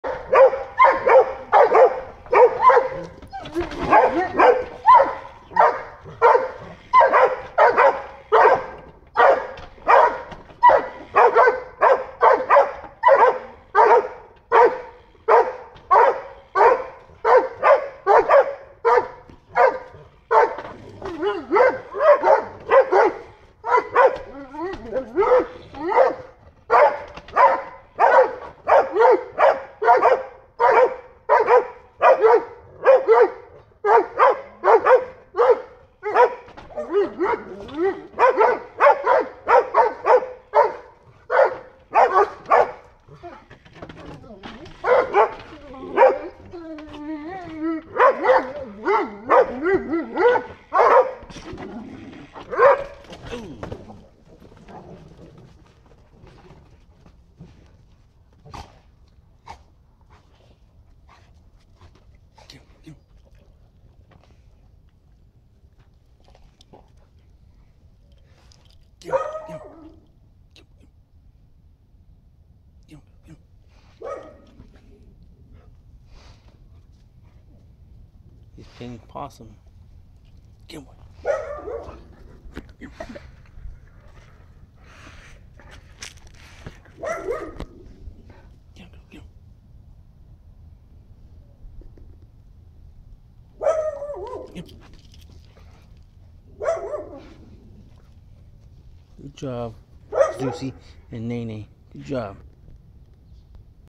Our dogs got a hold of a possum on the fence.
Our dogs get ahold of a possum on a fence.